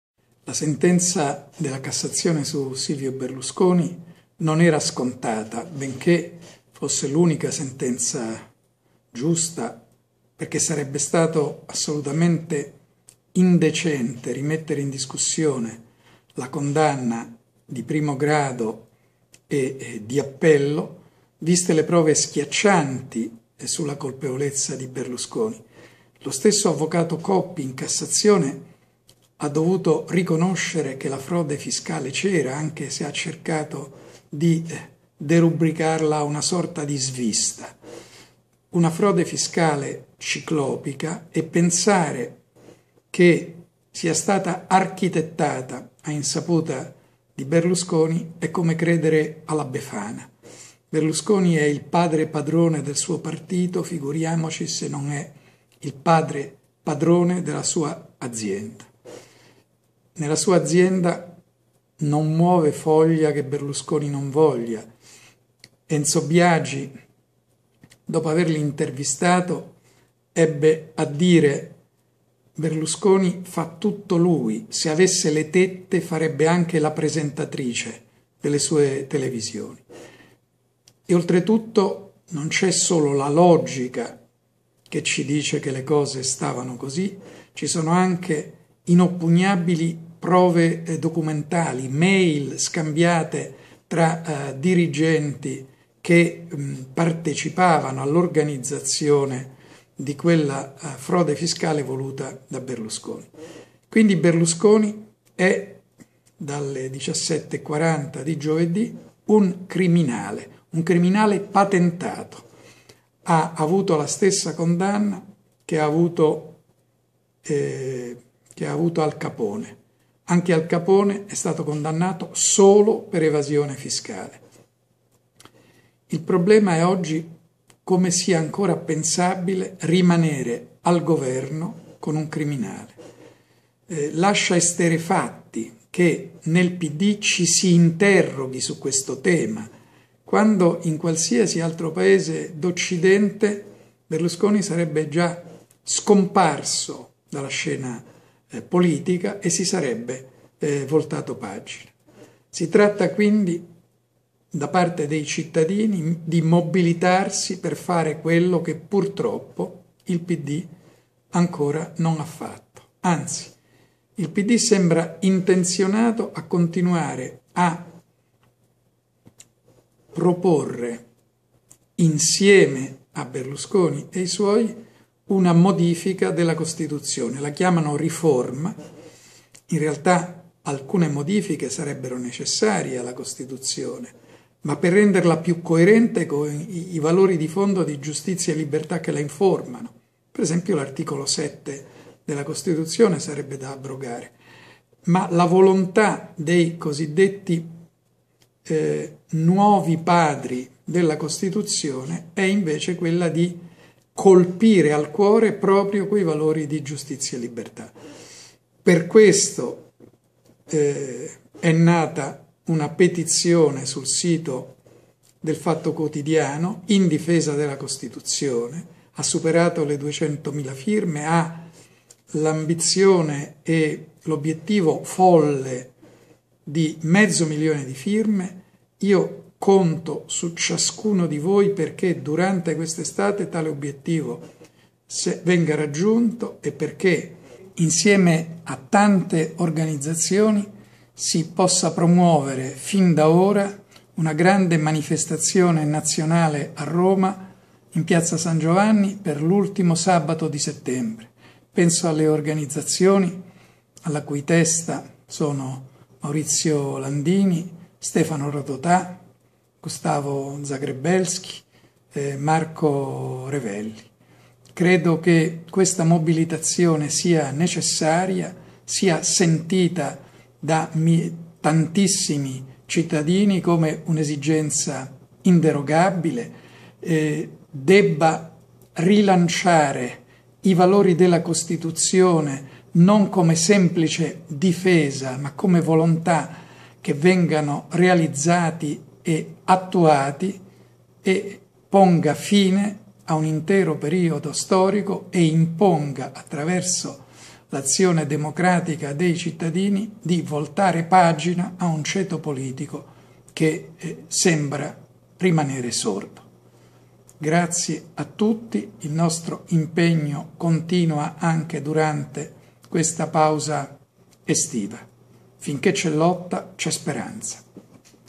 Flores d'Arcais: Cambiare la Costituzione con il Delinquente di Arcore? A settembre tutti in piazza!
Con la prima condanna definitiva per il Delinquente di Arcore si è aperto uno scontro decisivo tra gli unici due veri partiti esistenti in Italia: il partito della legalità e il partito dell'impunità, il cui obiettivo di fondo è lo smantellamento della Costituzione. Occorre mobilitarsi sin da ora per una grande manifestazione a Roma, il 28 settembre, per la legalità e la Costituzione. Leggi anche l'editoriale di Paolo Flores d'Arcais su